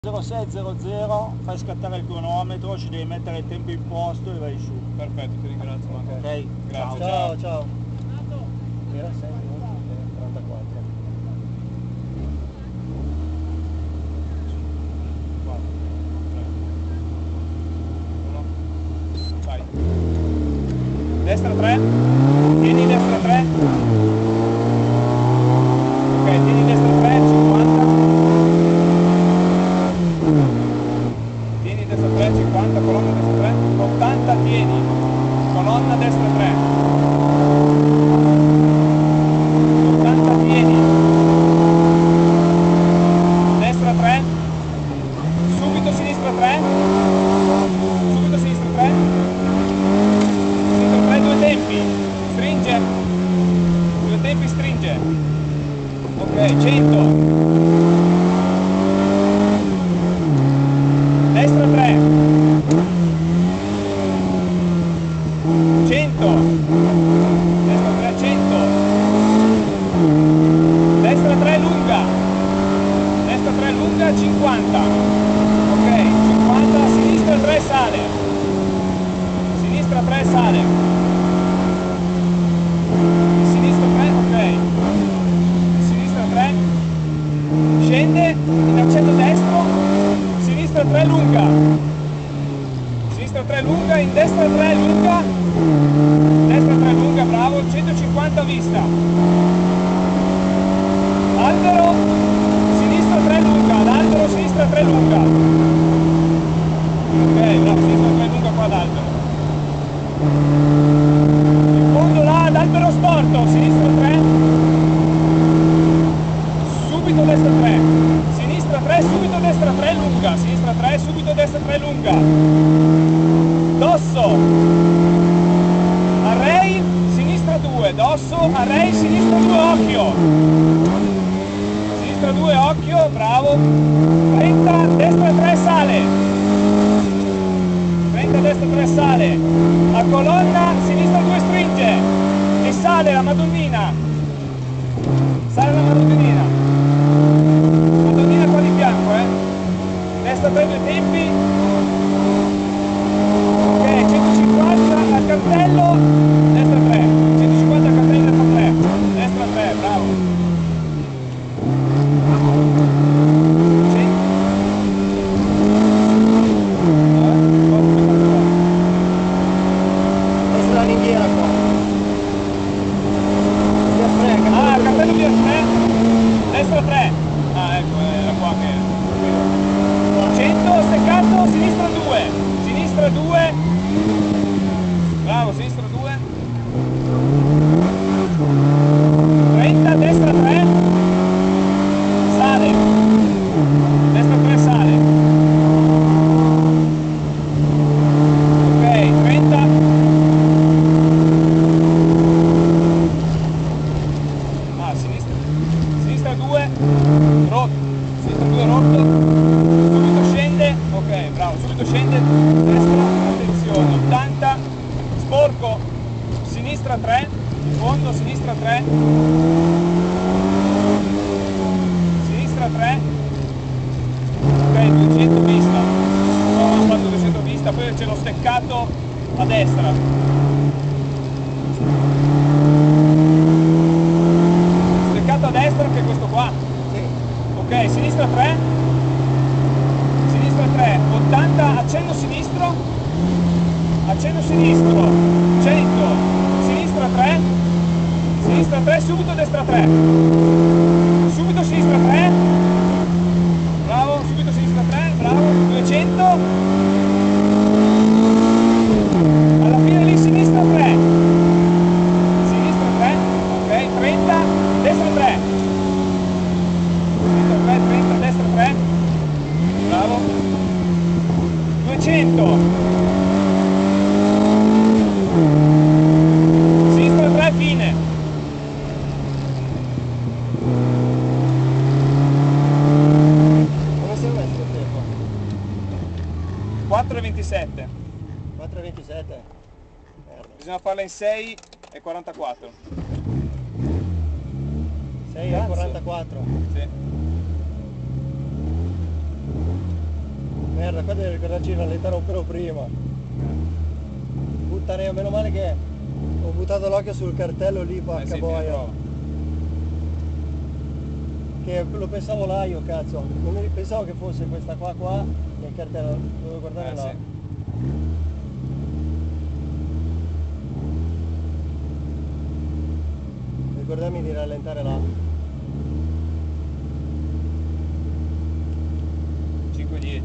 Memorial Conrero 2012 P.S. 1 Vialfrè.AVI
Memorial Conrero 2012 P.S. 1 Vialfrè
Fiat 127 Giannini NPS
Equipaggio Binati Marco - De Lucia Sandro
1° Classificati di Classe
15° Assoluti